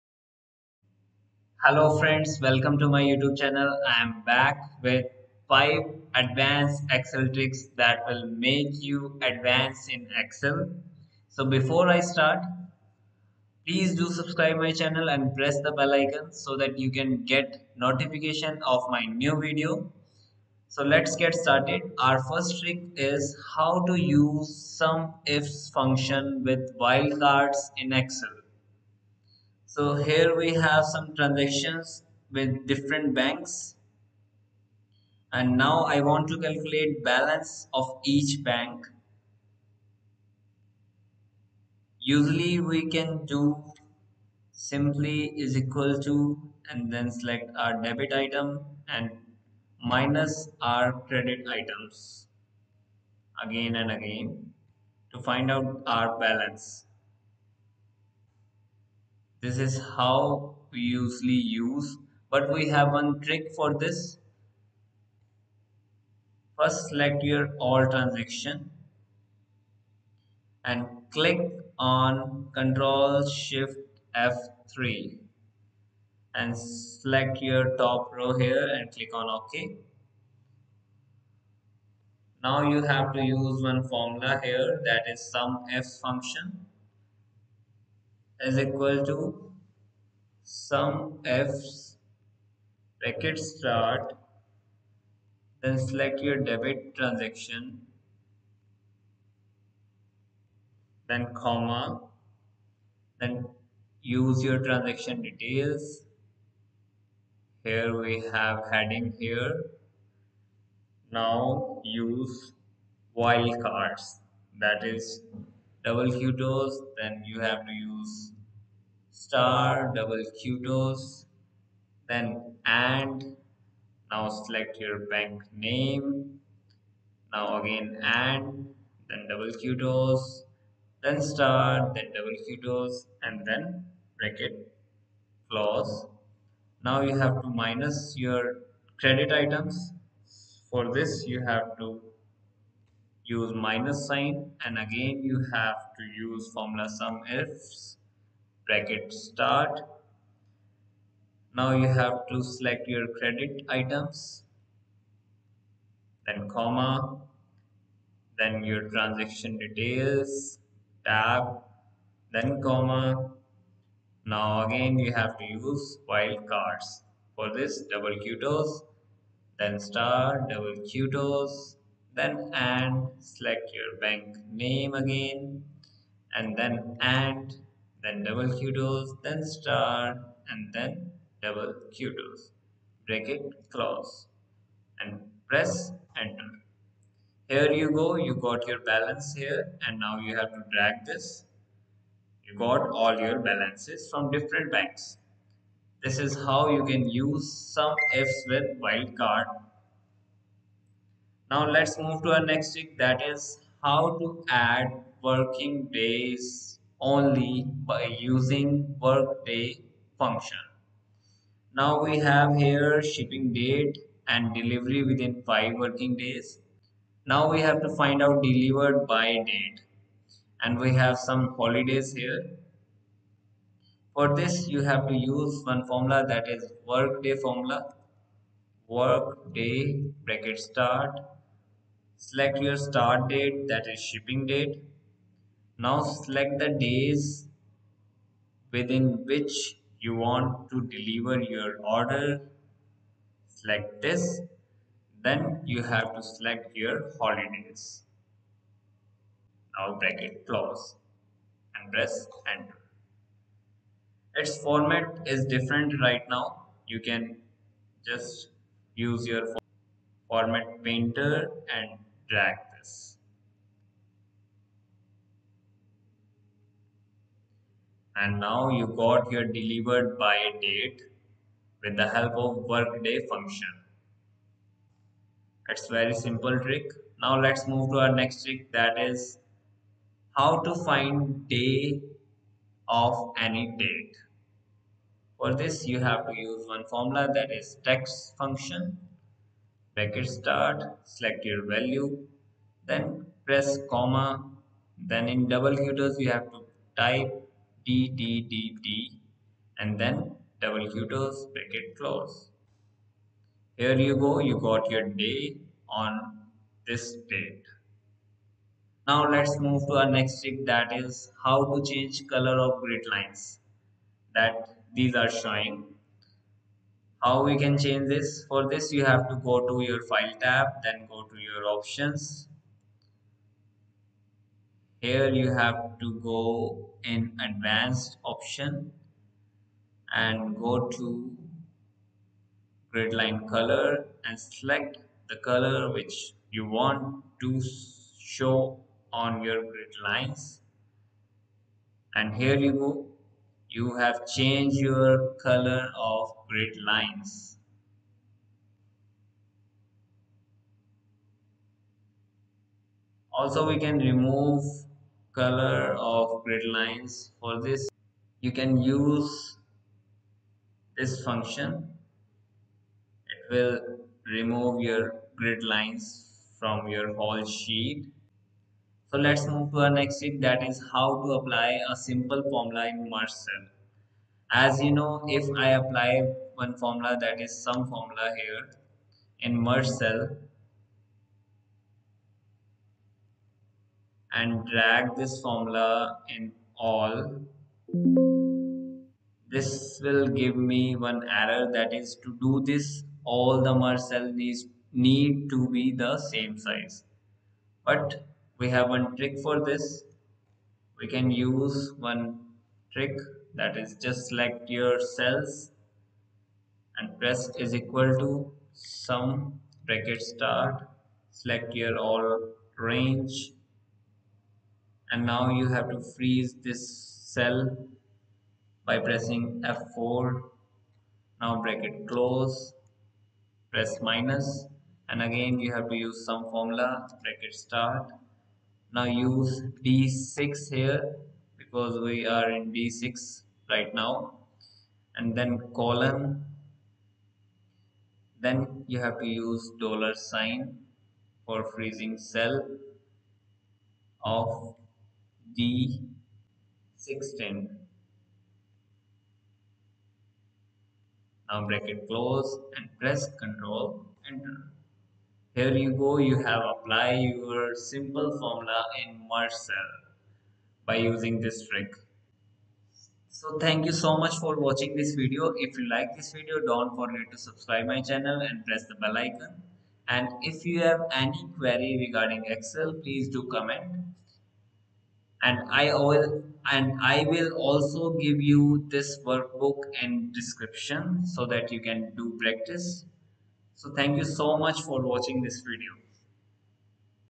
5 Advanced Excel Tricks to impress your Boss | Learn Ms-Excel Shortcuts and Formulas
In this Video, you will learn 5 excel tricks that will make you advanced in Excel.